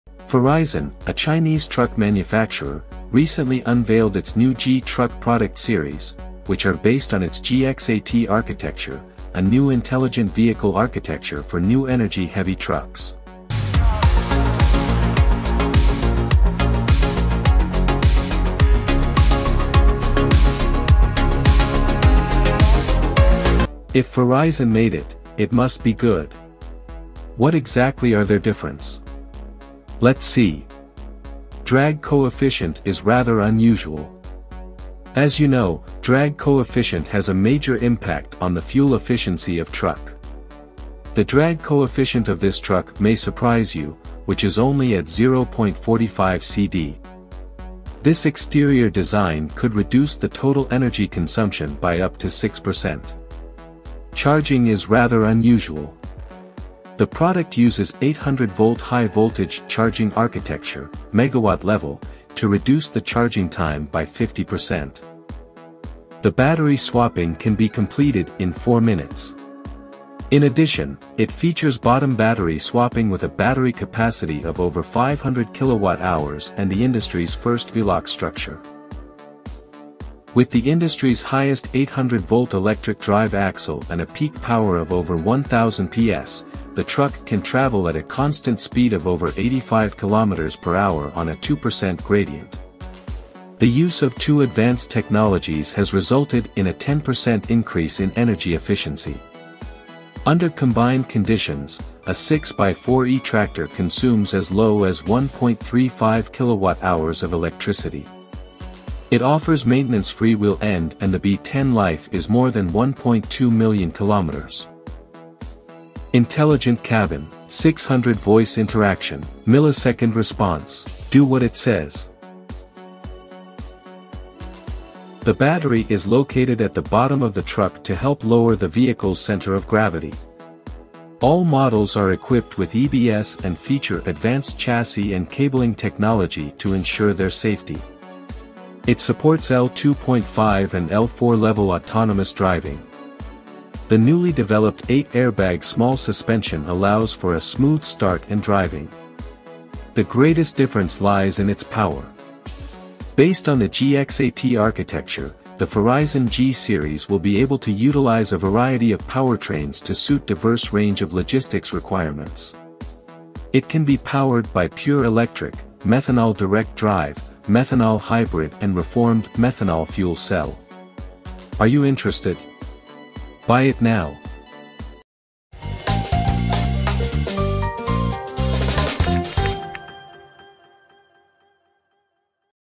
Farizon G Product Series, What Makes Them So Different?
Farizon, a Chinese truck manufacturer, recently unveiled its new G Truck Product Series, which are based on its GXA-T architecture, a new intelligent vehicle architecture for new energy heavy trucks.

If Farizon made it, it must be good!”
What exactly are their difference?   Let’s see.

Drag coefficient is rather unusual.
As you know, drag coefficient has a major impact on the fuel efficiency of truck. The drag coefficient of this truck may surprise you, which is only at 0.45Cd.  This exterior design could reduce the total energy consumption by up to 6%.

Charging is rather unusual.
The product uses 800 volt high voltage charging architecture (Megawatt Level) to reduce the charging time by 50%. In addition, It features bottom battery swapping with a battery capacity of over 500kWh and the industry's first V-lock structure. The battery swapping can be completed in 4 minutes.

With the industry's highest 800-volt electric drive axle and a peak power of over 1,000 Ps, the truck can travel at a constant speed of over 85 km/h on a 2% gradient.

The use of two advanced technologies has resulted in a 10% increase in energy efficiency. Under combined conditions, a 6*4 e-tractor consumes as low as 1.35 kWh of electricity. It offers maintenance-free wheel end and the B10 life is more than 1.2 million km.

Driving Experience is unusual.
Intelligent Cabin
600 voice Interaction
Millisecond Response
Do what it says.

The battery is located at the bottom of the truck to help lower the vehicle's centre of gravity.  All models are equipped with EBS and feature advanced chassis and cabling technology to ensure their safety. It supports L2.5 and L4 level autonomous driving. The newly developed 8 airbag small suspension allows for a smooth start and driving.

The greatest difference lies in its power. Based on the GXA-T architecture, the Farizon G Series will be able to utilize a variety of powertrains to suit diverse range of logistics requirements.  It can be powered by pure electric, methanol direct drive, methanol hybrid and reformed methanol fuel cell.  Are you interested?